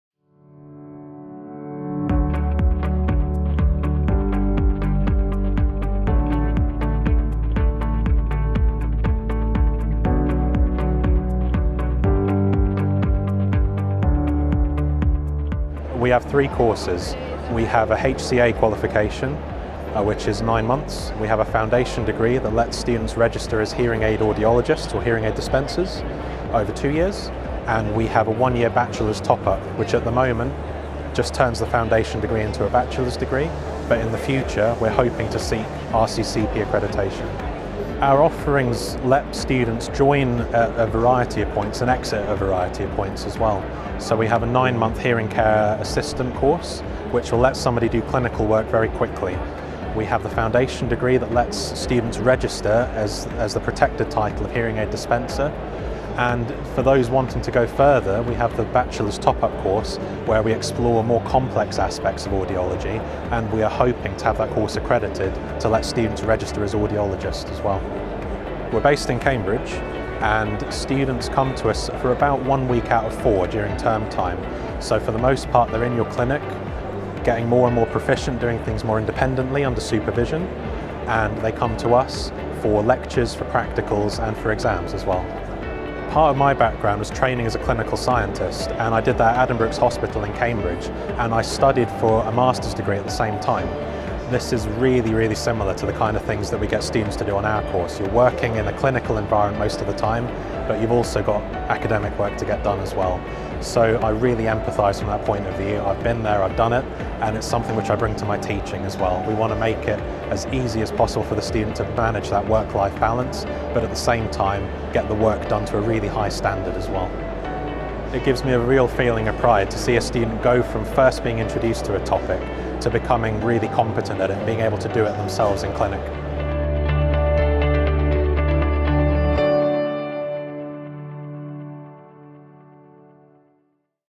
Hearing Aid Audiology Course
If you are looking to better support your customers with their Hearing requirements, you need a qualified and compassionate team of experts. Our courses provide a Hearing Assistant qualification through to Hearing Aid Dispenser training, delivered online and as part of residential in Cambridge, to fit in around your business.